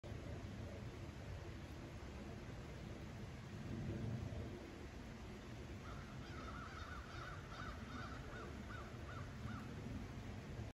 First snow!
Nov 3rd, 2021 First snowfall of the year for Owego, NY.